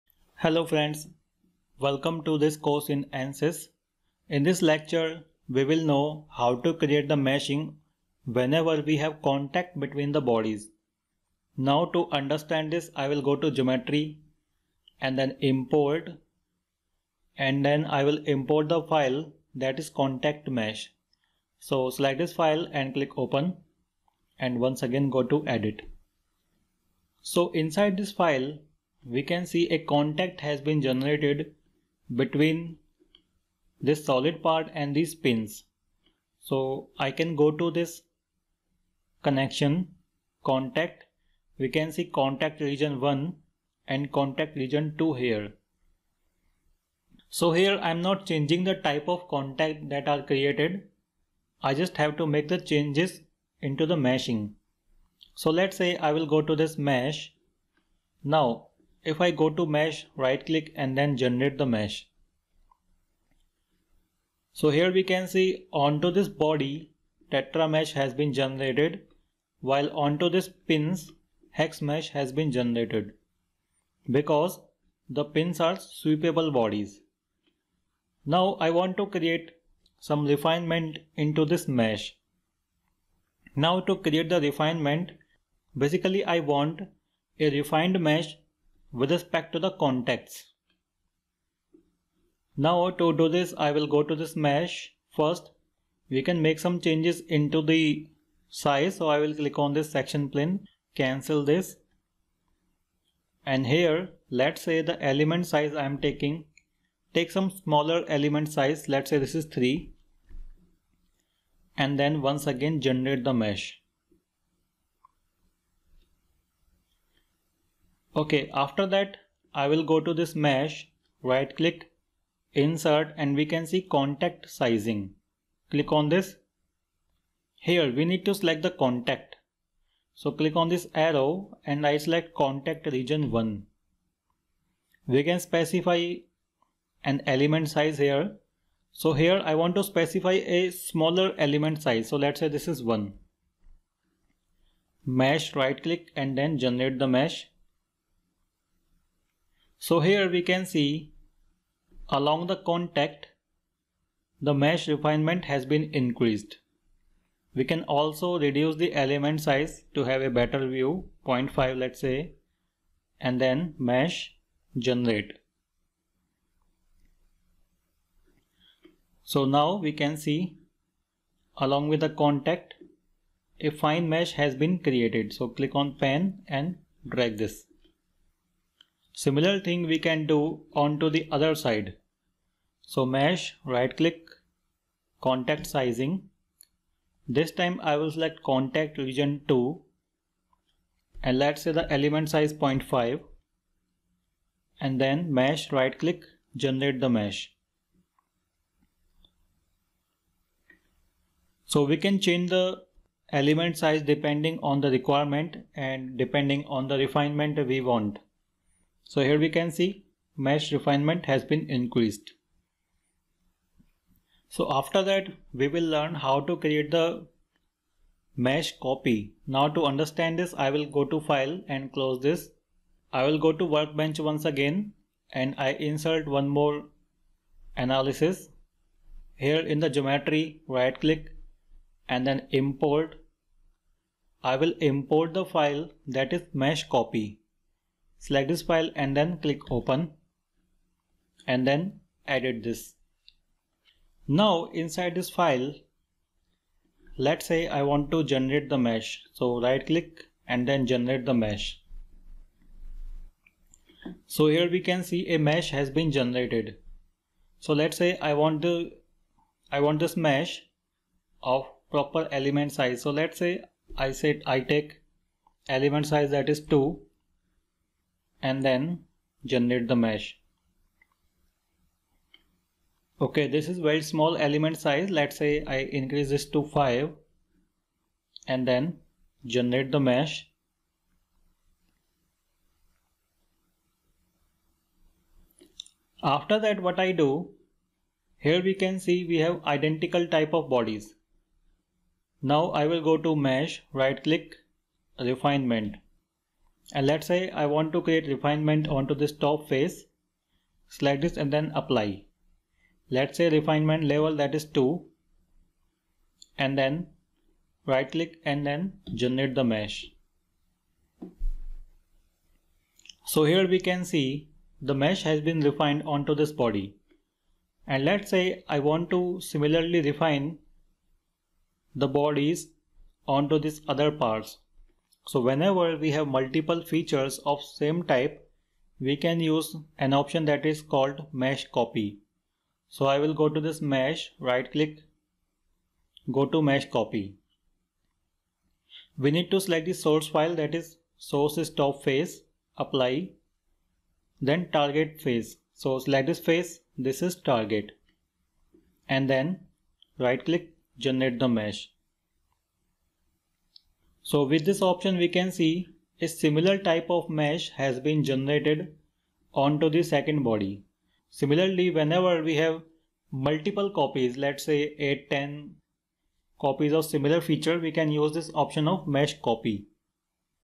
ANSYS workbench | 09 Mesh Copy
in this lecture, you will learn how to copy mesh

LS Dyna

Abaqus

Hypermesh

NX

CREO

Ansys mechanical APDL

Ansys Structural

Siemens Femap Nastran